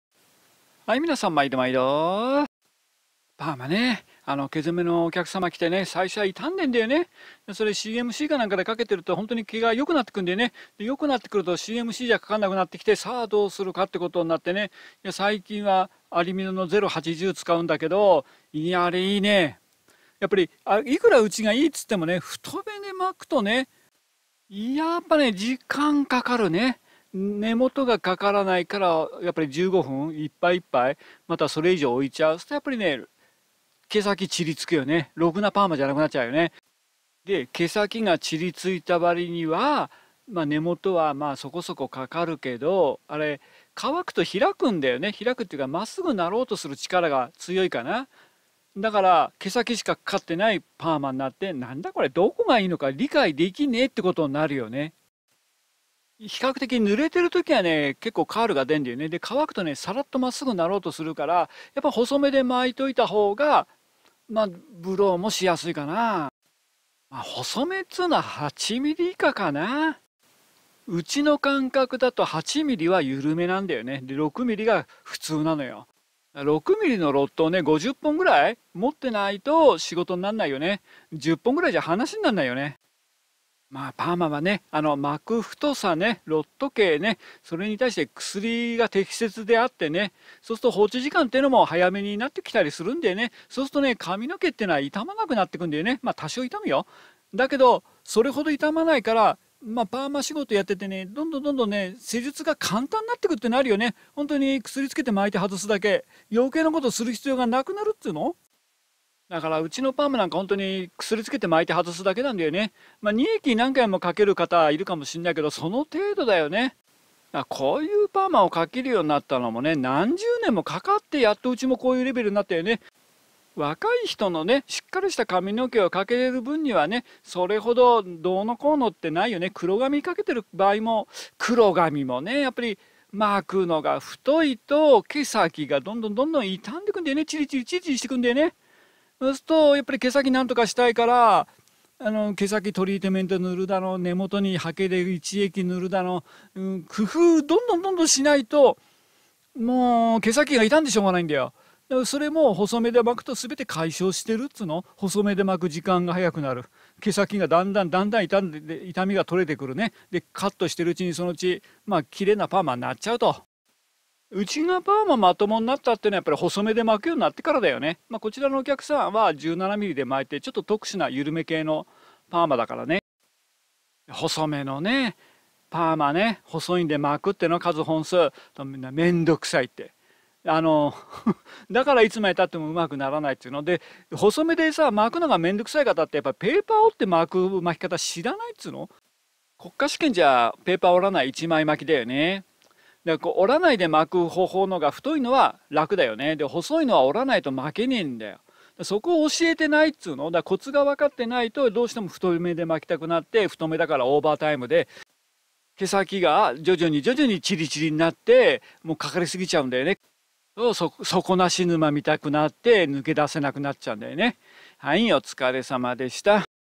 聞くだけでパーマが上手くなる与太、いい薬も使い方間違えると悪い薬になる
ちょっと細めで巻く、この簡単な事が出来ずに何十年も損をしてきた